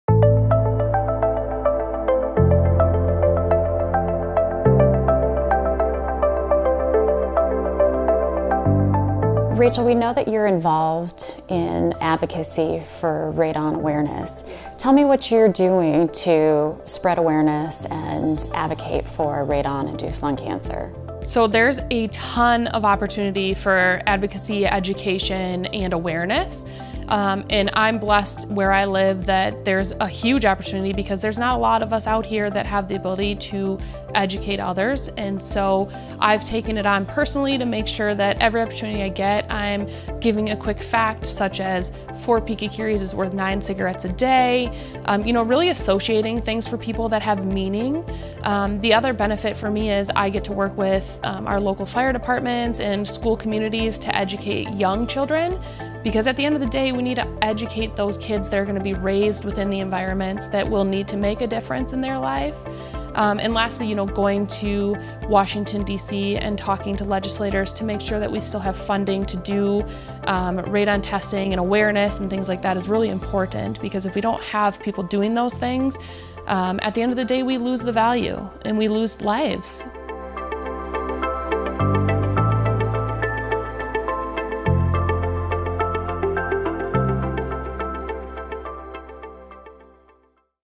Test4Radon Episode 5: Advocacy for radon testing
Watch Rachael Malmberg spreading awareness about Radon testing and advocating for radon induced lung cancer.

** Cliquez sur CC et * pour les sous-titres en Français **
** Klicken Sie auf CC und * für Untertitel in Deutsch **
** Нажмите СС и * для субтитров на Русском **